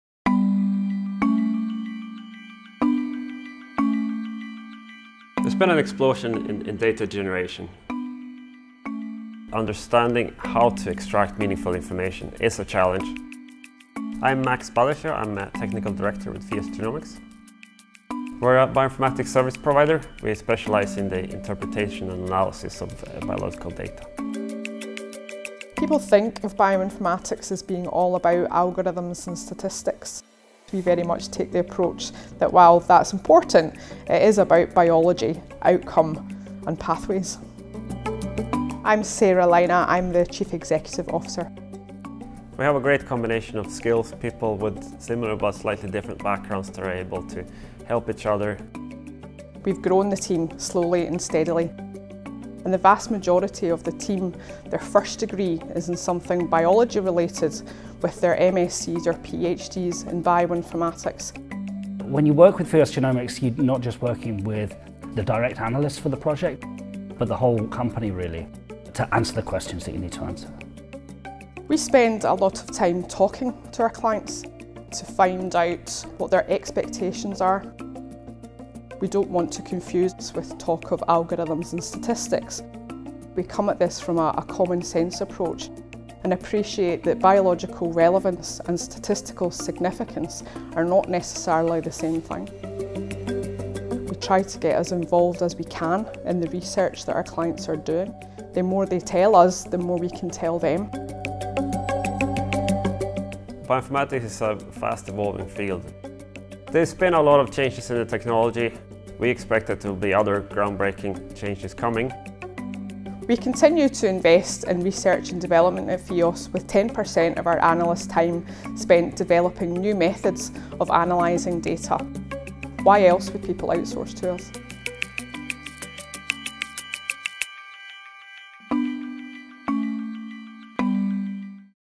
We are Fios Genomics
Understanding how to extract useful information from data is a difficult task and at Fios Genomics we are well aware that the most important thing for our clients is the biology outcome of their research.

Our team comes from a variety of different backgrounds: biology, statistics, and bioinformatics.
We run the extra mile, and continue to invest in R&D, in new analysis methods and techniques to help biotechnologies and pharmaceutical companies in their research.